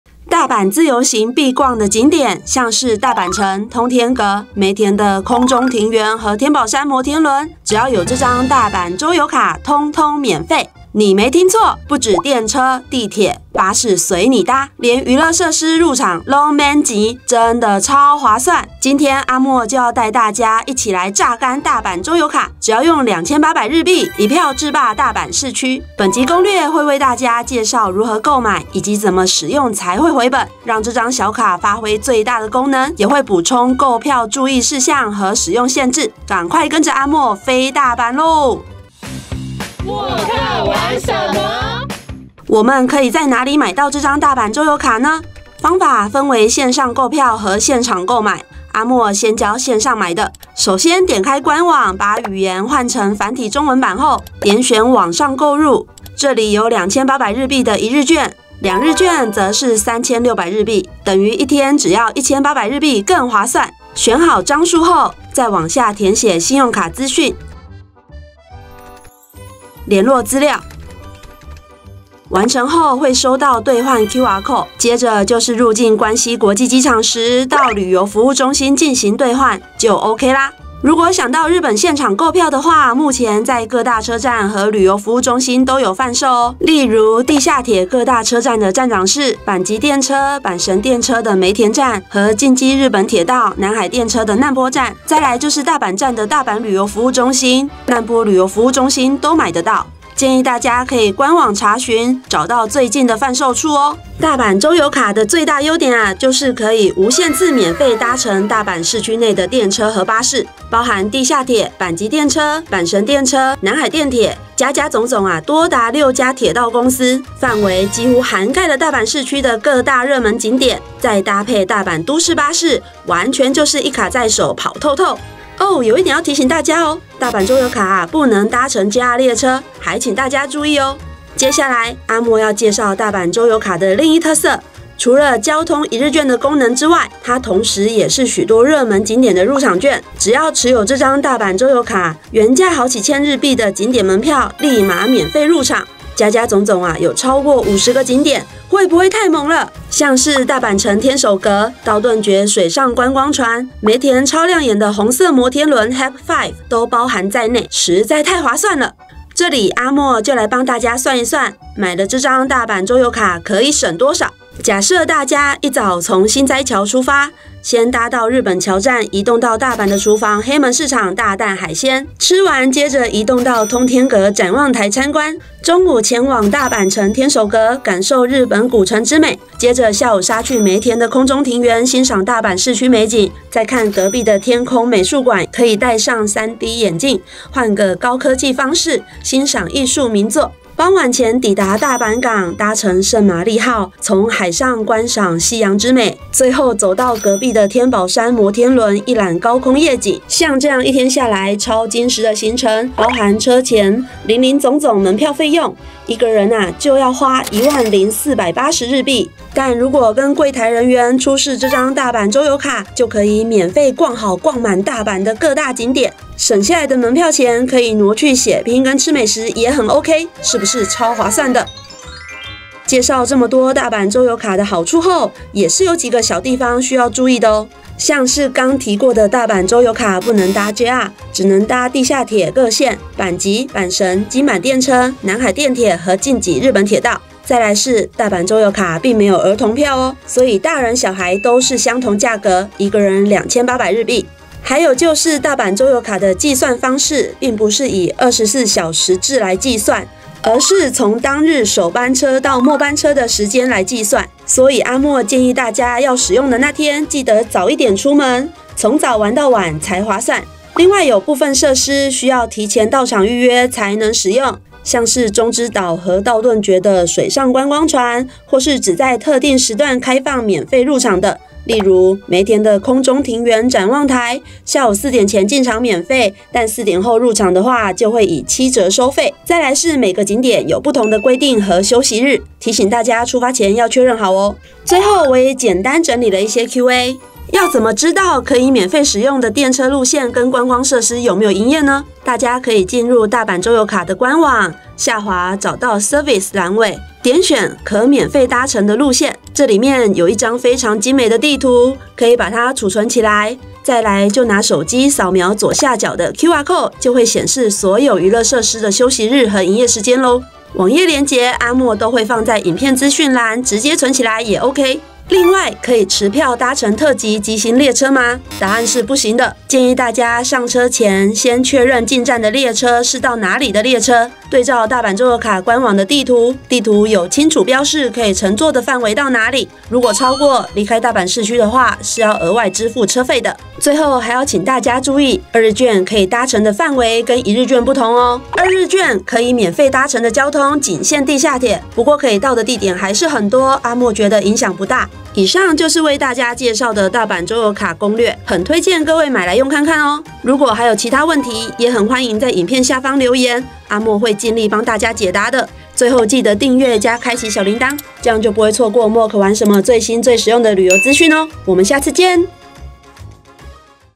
大阪周遊卡→通通免費！使用全攻略｜大阪自由行必備｜交通‧必玩景點｜通天閣·大阪城天守閣·梅田空中庭園·天保山摩天輪👈MOOK玩什麼
⌚本集章節
00:00📍前言開始
00:43📍購票教學
01:44📍使用範圍
02:39📍大阪1日遊排行程
03:43📍注意事項

【特別な感謝】
誠摯感謝本集攻略影片由以下單位協力
この動画は以下の協力で完成いたしました

〔credit〕
腳本企劃 步小心（林于心）  Chiu,Ming-Chen
攝影 拉拉 Chiu,Ming-Chen
剪輯 劉曉天當代影像
配音 拉拉

🟡更多資訊可參考MOOK新書《東京攻略完全制霸2023~2024》